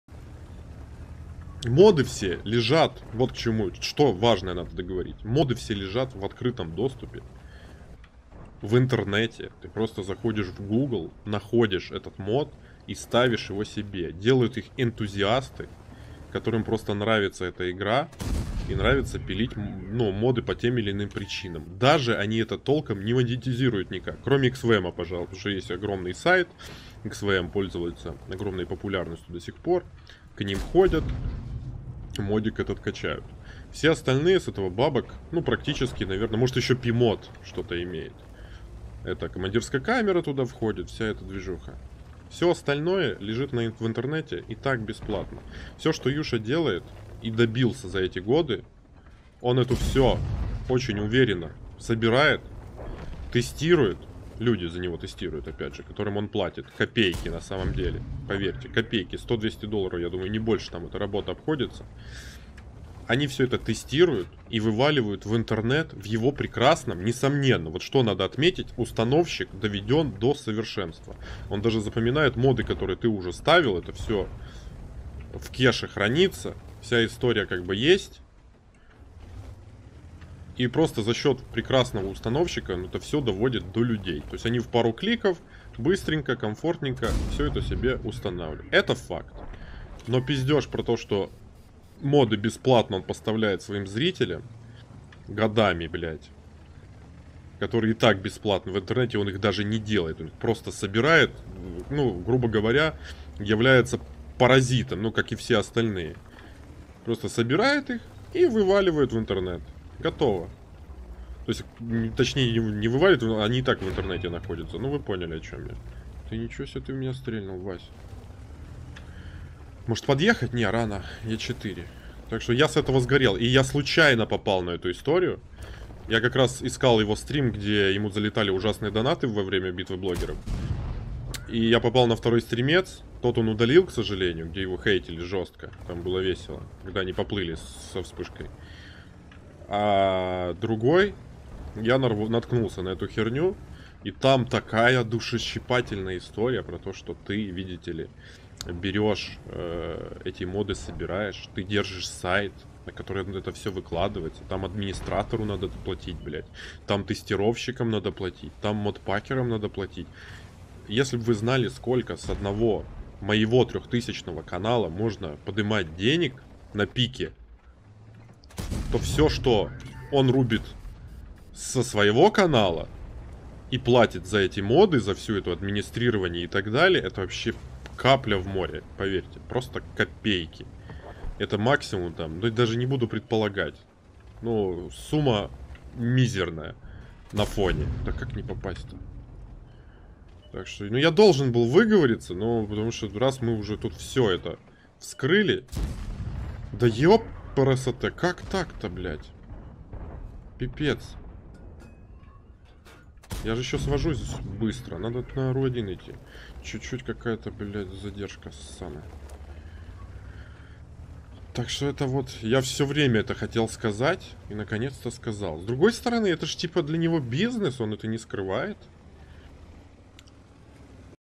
МОДПАК КАКОЙ ЛУЧШЕ # СКОЛЬКО НА ЭТОМ ЗАРАБАТЫВАЮТ
Во время прошлой ББ и ББ 2021 Никитос снова вернулся, чтобы поддержать Амвея на Битве блогеров.
 В данной нарезке он рассказывает о том, почему плохо относится к Юше, откуда Юша Протанки берёт моды, а так же, как дорого ему это обходится.